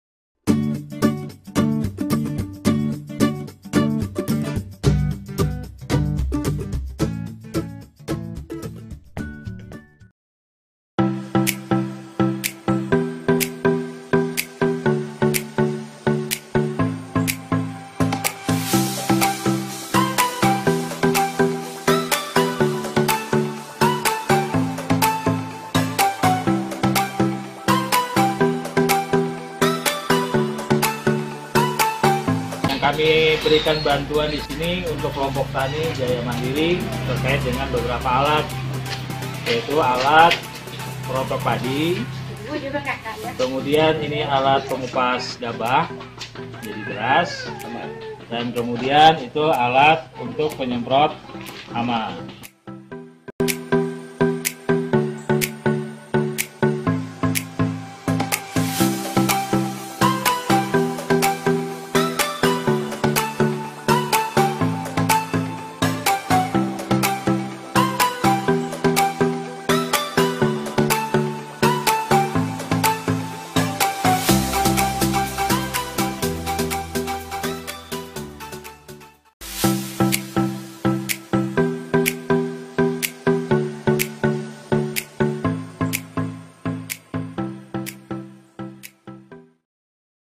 Pengabdian kepada masyarakat pemberdayaan Kelompok Tani Jaya Mandiri
Pengabdian kepada masyarakat pemberdayaan Kelompok Tani Jaya Mandiri di Kelurahan Batu Layang Kecamatan Pontianak Utara, Kota Pontianak, Kalimantan Barat